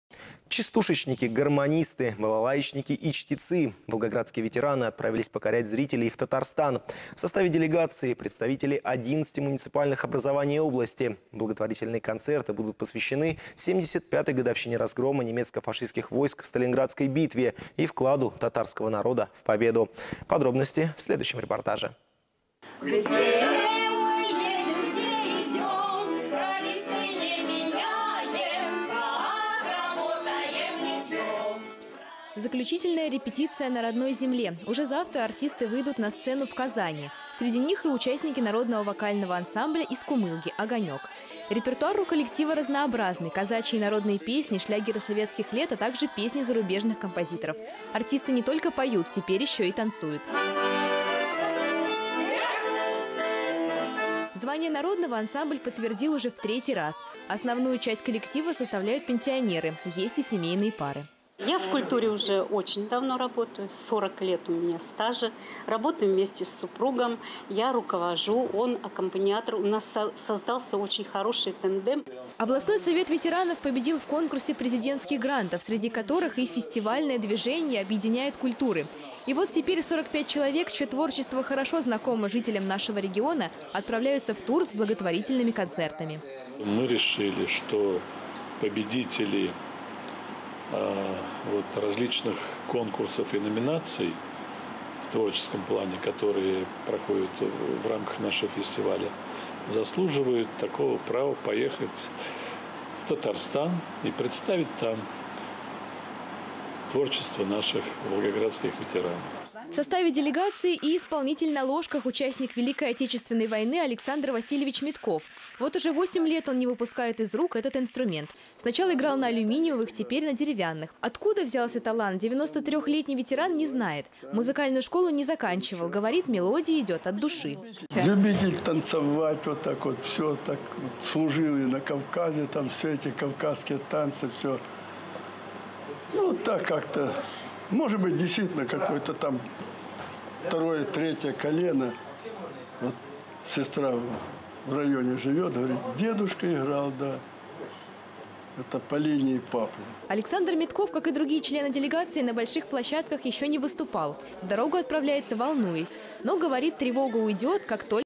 Волгоградские ветераны отправились с благотворительным концертом в Татарстан
Частушечники, гармонисты, балалаечники и чтецы - волгоградские ветераны отправились покорять зрителей в Татарстан. В составе делегации - представители 11 муниципальных образований области. Благотворительные концерты будут посвящены 75-й годовщине разгрома немецко-фашистских войск в Сталинградской битве и вкладу татарского народа в Победу.